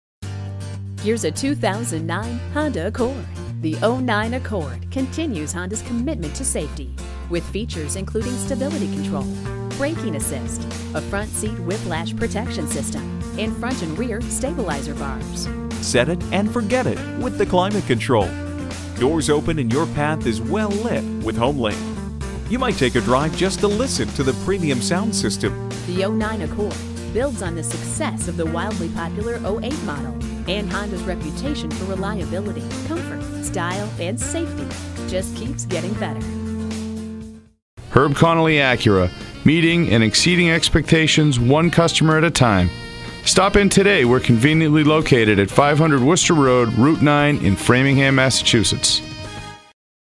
2009 Honda Accord Framingham Natick Marlborough MA, MA A002739A - SOLD
Year: 2009
Make: Honda
Model: Accord
Trim: EX-L
Engine: 3.5L V6 SOHC i-VTEC 24V
Transmission: 5-Speed Automatic with Overdrive
Interior: Black
Mileage: 117515
Stock #: A002739A
VIN: 1HGCP36869A049493

Address:
500 Worcester Road Route 9 E